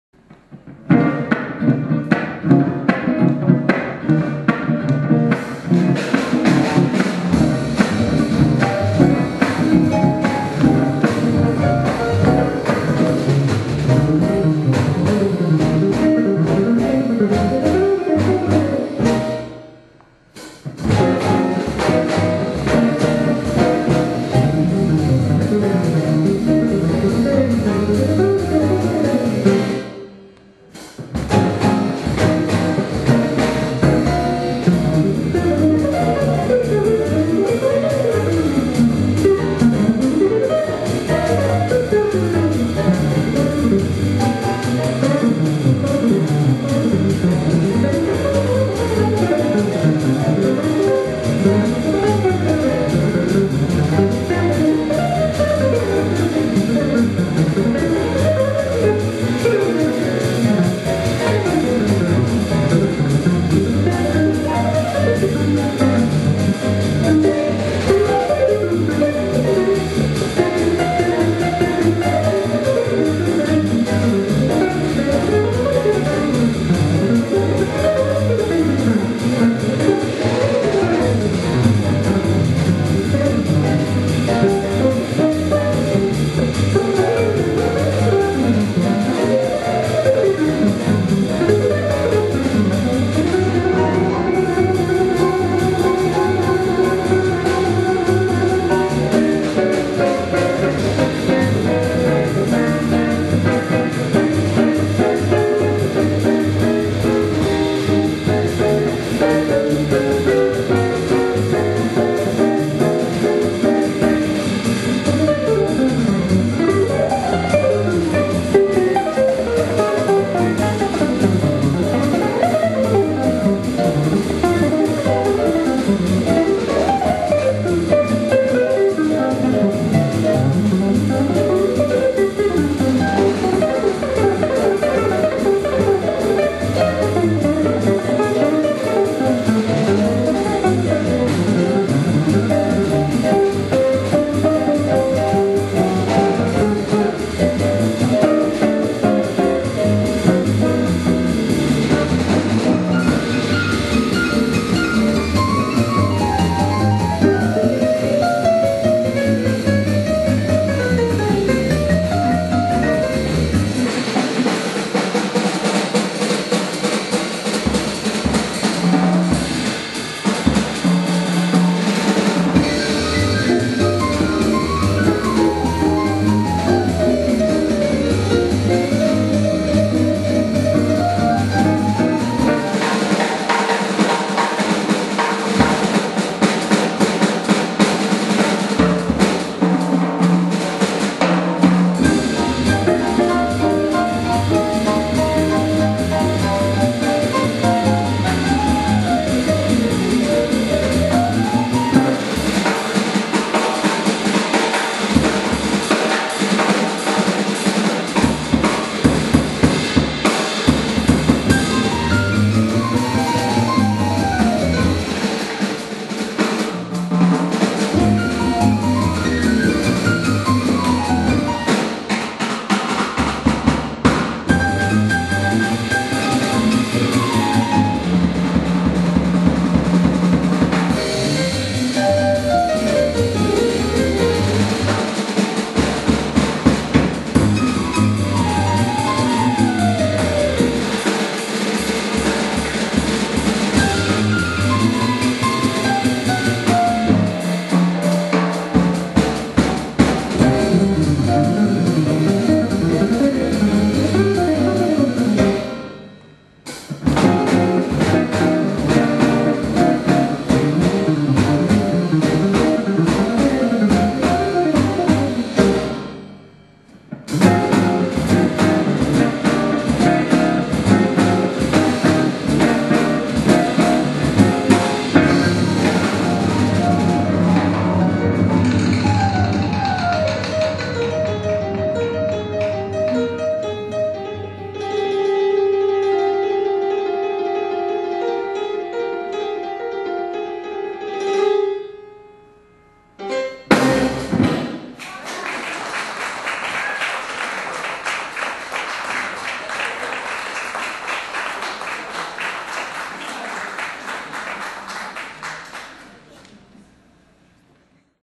Denis Chang au Moe's
Denis Chang au Moe's durant le FestiJazz de Mont Tremblant.Eric Dew à la batterie, Paul VanDyk à la contrebasse, Ivan Garzon à la guitare rythmique et David Ryshpan au piano